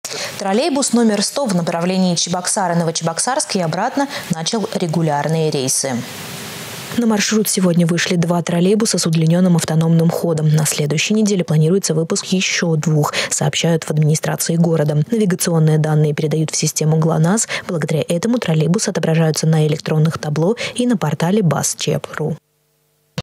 Троллейбус №100 начал регулярные рейсы
Троллейбус №100 в направлении Чебоксары – Новочебоксарск  и обратно начал регулярные рейсы.